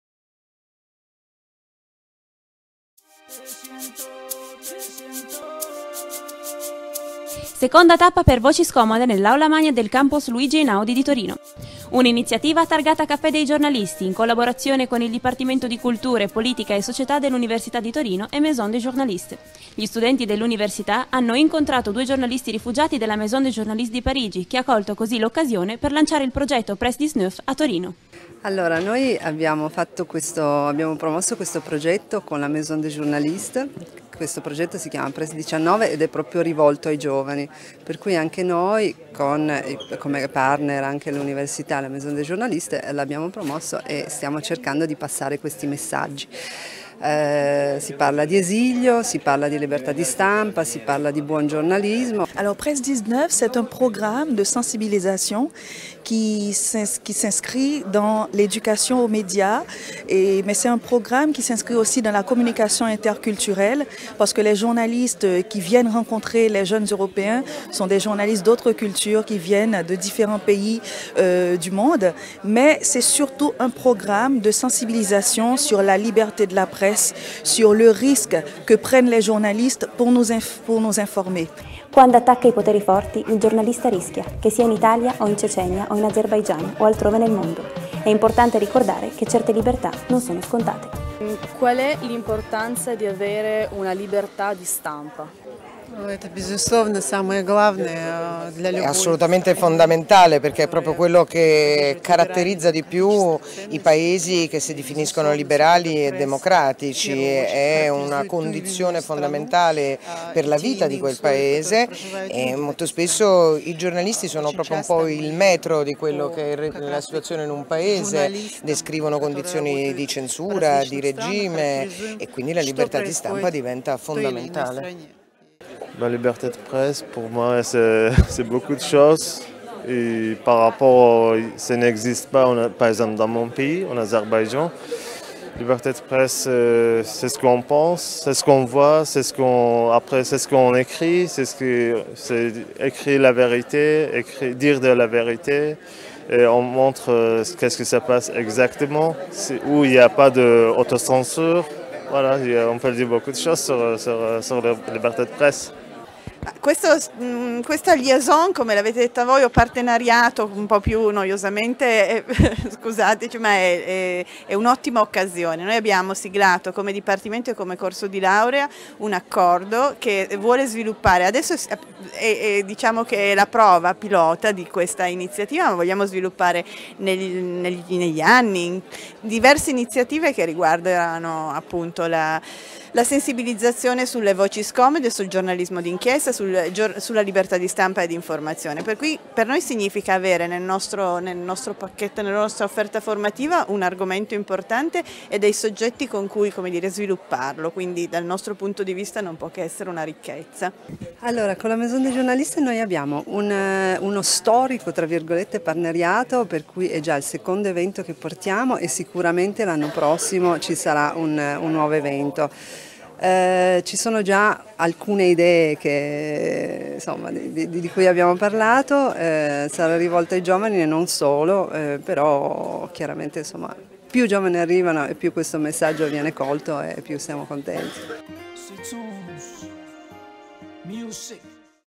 Presse 19 : Rencontre à l'Université de Turin - Italie
Vendredi 28 novembre 2014 - Turin (Italie)

La Maison des journalistes a lancé le programme Presse 19
avec les journalistes réfugiés Agil Khalilov (Azerbaïdjan) et Zara Mourtazalieva (Tchétchénie - Russie)

en collaboration avec le Caffè dei Giornalisti et l'Université de Turin

Reportage : Tekla Télévisions